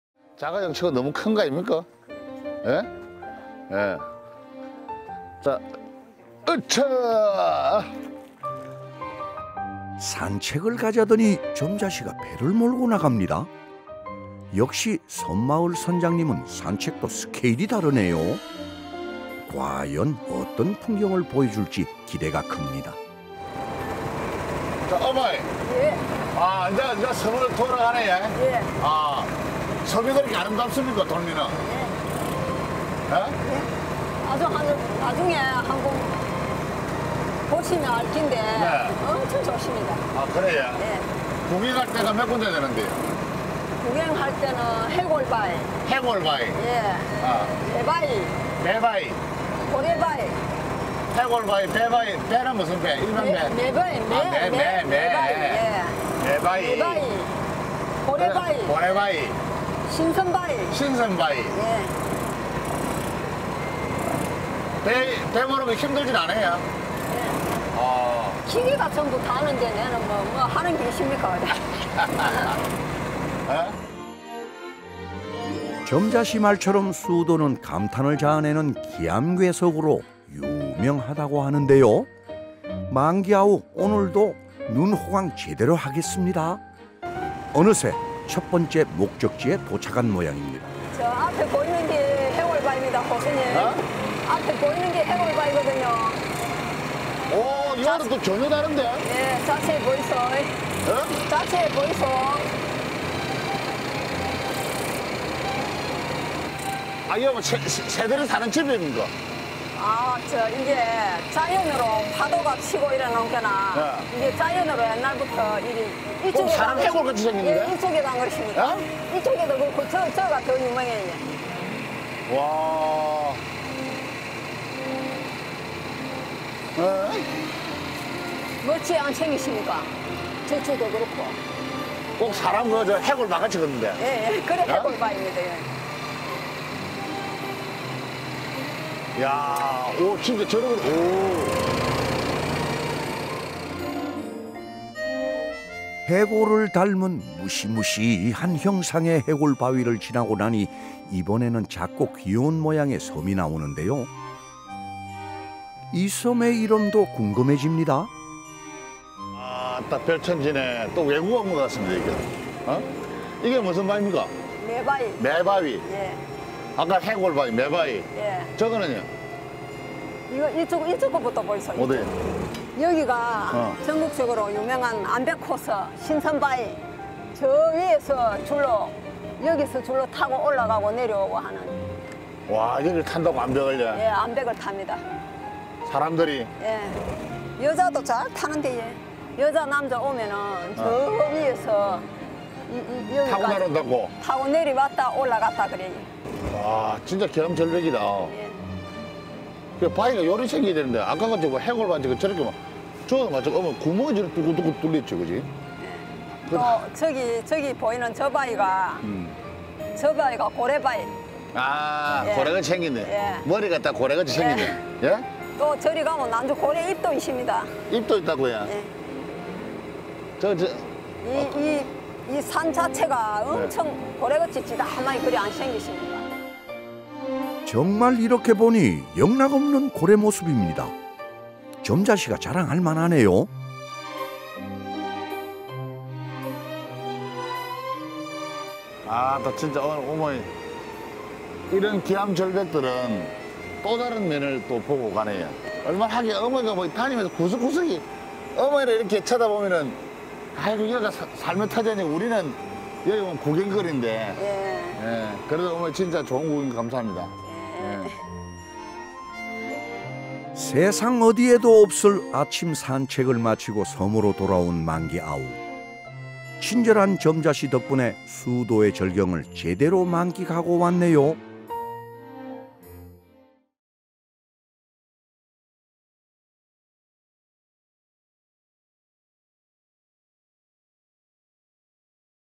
섬할매 선장님과 함께 멋진 풍경 보러갈래❓🌅ㅣ섬마을할매 시즌2
이게 섬마을 산책 클라스다🌟
멋진 경관과 함께하는 산책 💕💕
섬마을 선장님과 함께 떠나보자😘